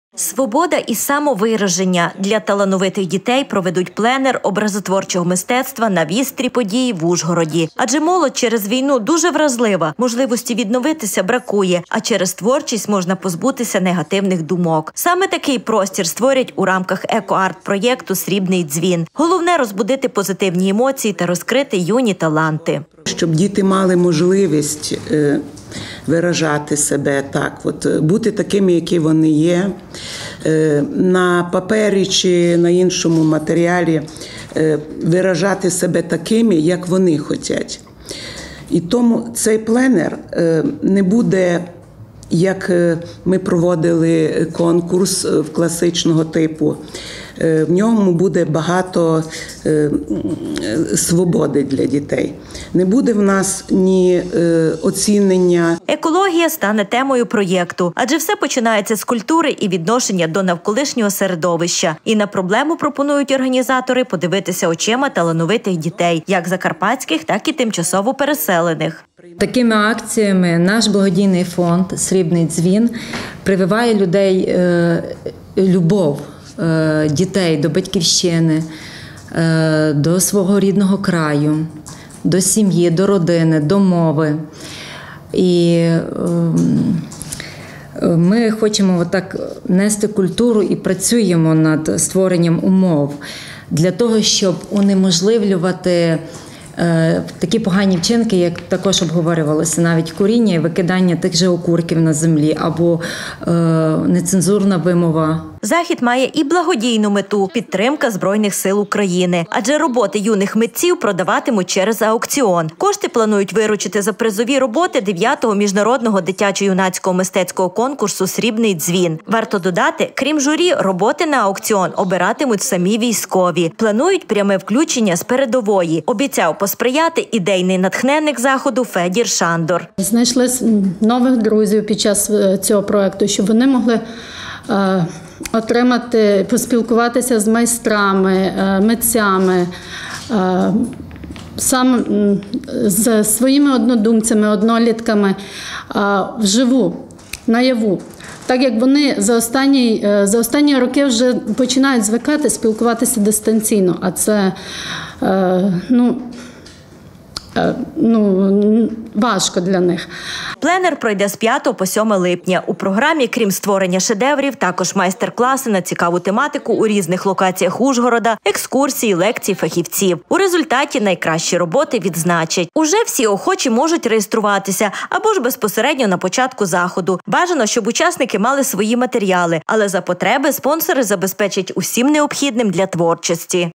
Благодійний екопленер
Пленер образотворчого мистецтва "На вістрі подій" проведуть в Ужгороді. Захід організують у рамках  ЕкоАртПроєкту  "Срібний Дзвін" для  закарпатських  юних  художників та дітей,  які тимчасово проживають у краї